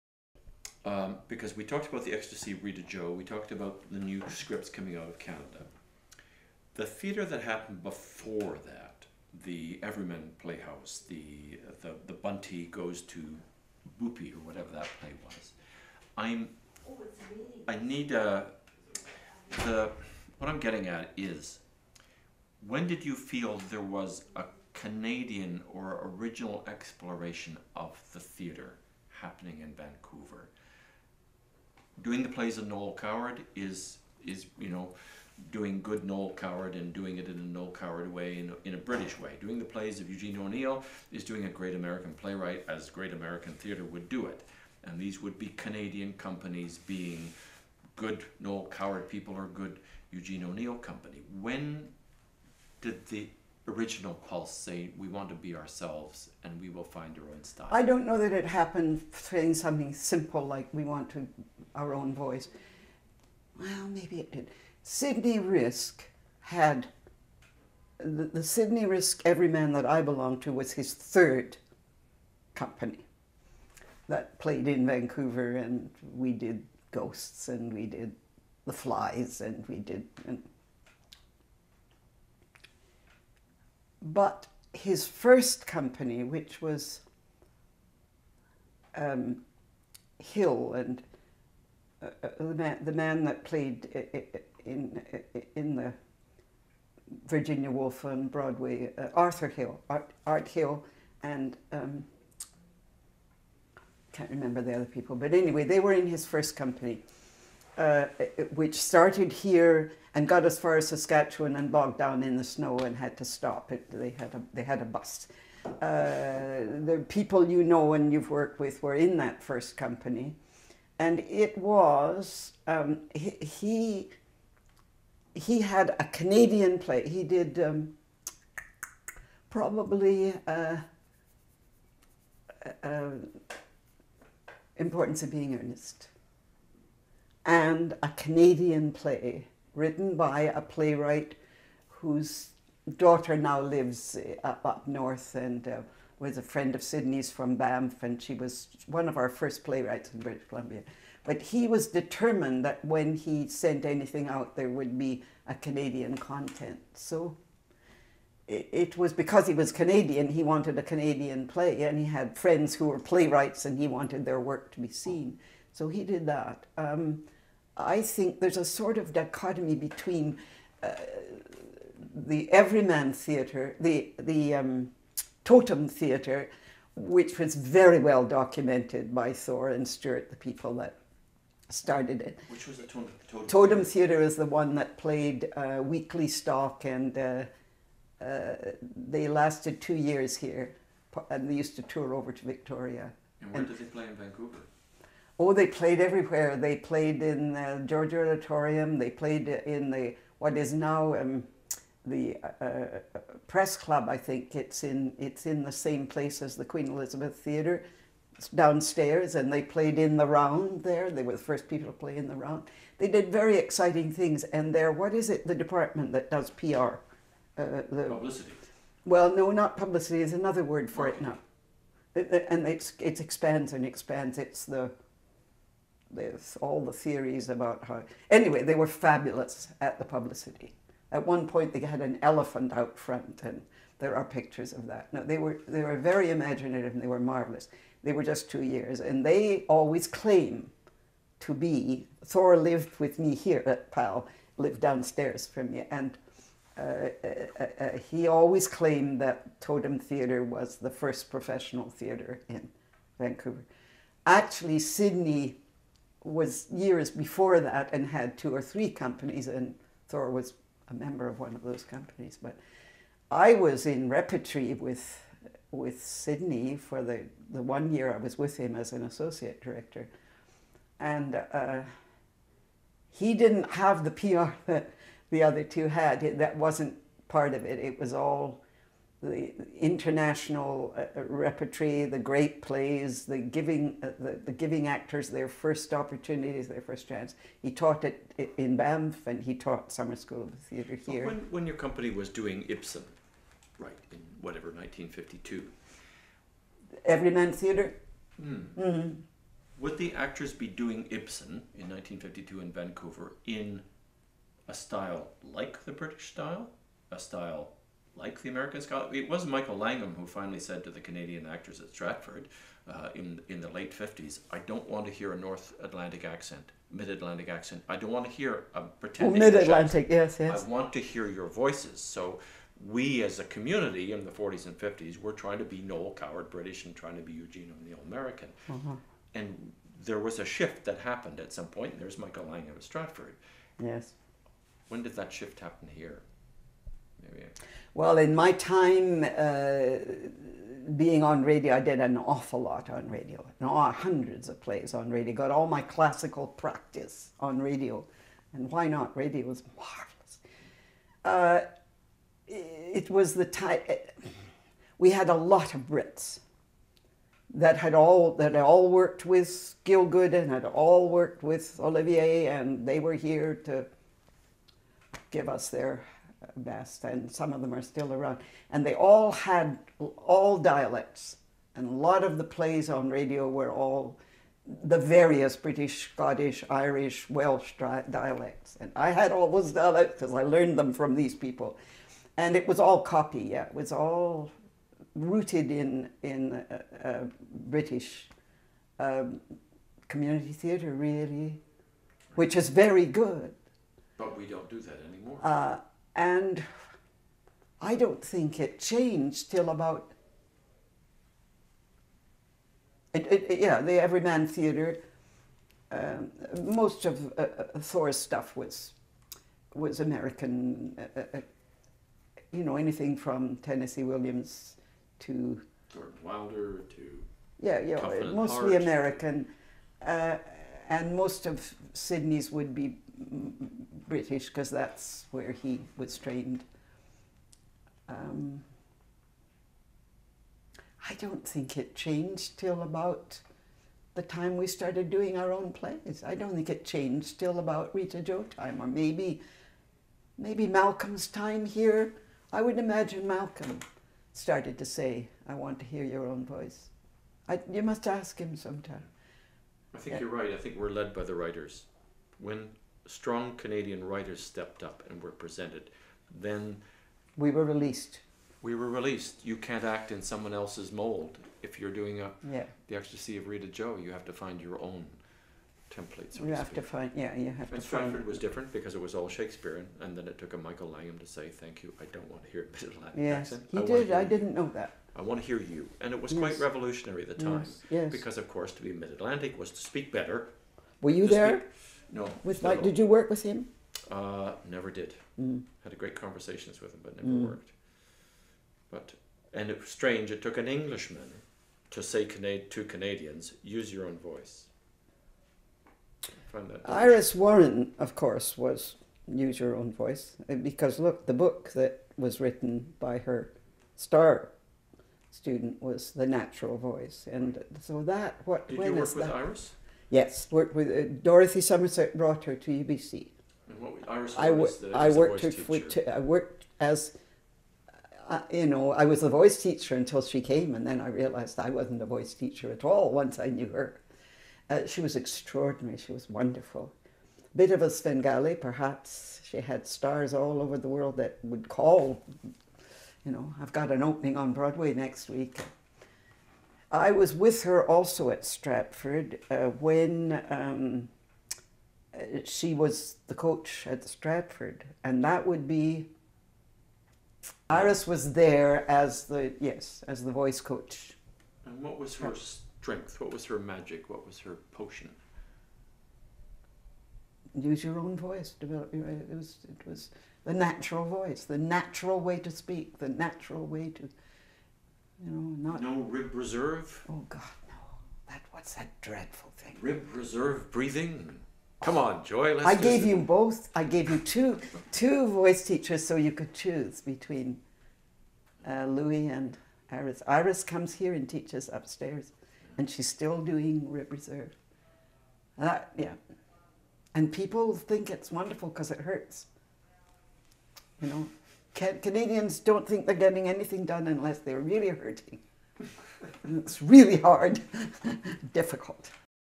Joy Coghill on early theatre in British Columbia, radio, and Iris Warren (Part 8 of 9)
Joy Coghill on early theatre in British Columbia (Everyman, Totem), radio, and Iris Warren.

R.H. Thomson asks Coghill when the distinctly Canadian voice emerged in West Coast theatres. Coghill recounts that before Sydney Risk created Everyman Theatre in Vancouver, he and Arthur Hill had founded a theatre company which presented Canadian content. Thomson and Coghill discuss the shift from imitating the British and the Americans to embracing a Canadian style. In the late 1950s, Michael Langham told the actors at the Stratford Festival to abandon the use of a Mid-Atlantic accent and to simply use their own voices. Coghill recalls how she began her career doing plays on radio and imitating the dialects used by British actors that came over to Canada. In her opinion, it changed when Canadians started performing Canadian plays. “When strong Canadian writers stepped up -- we were released.”

It was also the philosophy of legendary voice coach Iris Warren to use your own, natural voice. Coghill reminisces on working with Warren at both the University of British Columbia and at the Stratford Festival.

Interviewed by R.H. Thomson on December 14, 2010 in Vancouver, B.C.

Filming location courtesy of Performing Arts Lodges (PAL) Vancouver.